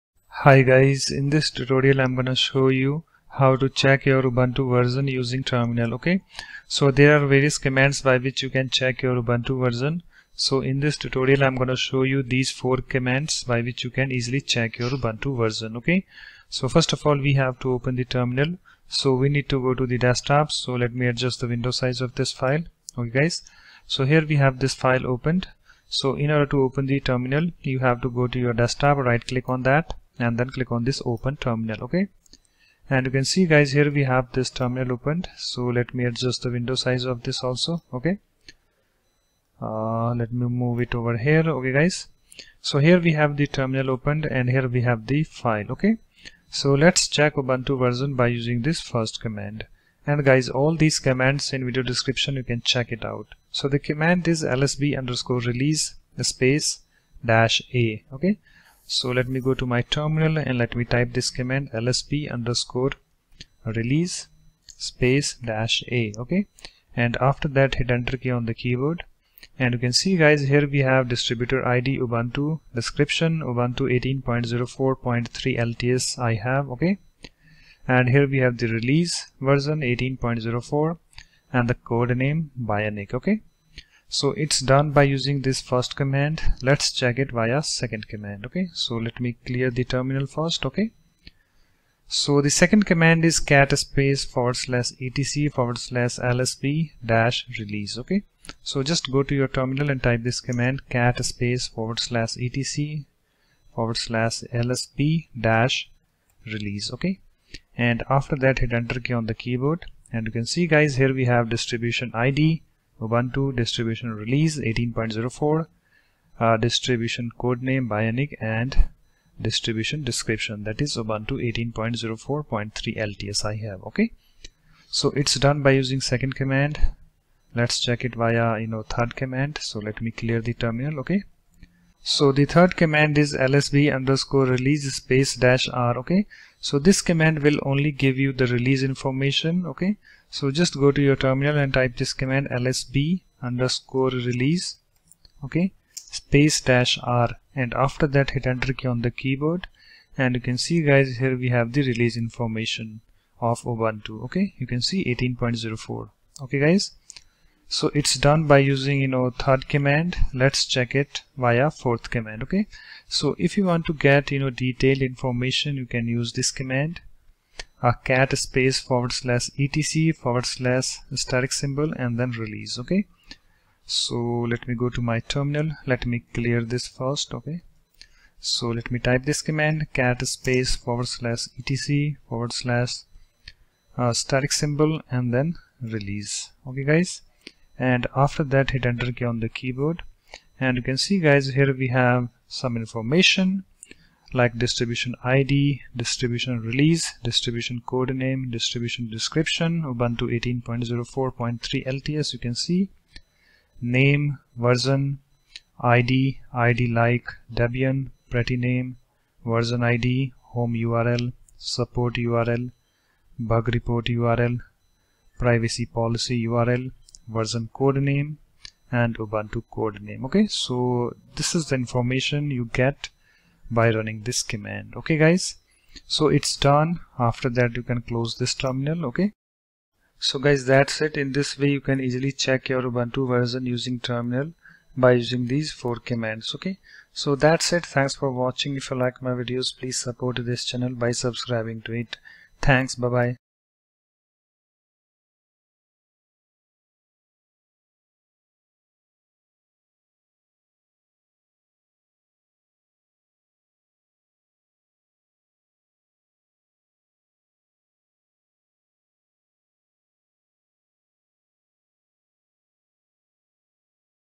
How to Check Ubuntu Version Using Terminal?
There are various commands by which you can check your Ubuntu Version:-

1) lsb_release -a

2) cat /etc/lsb-release

3) lsb_release -r

4) For More detailed information, run below command:-

   cat /etc/*release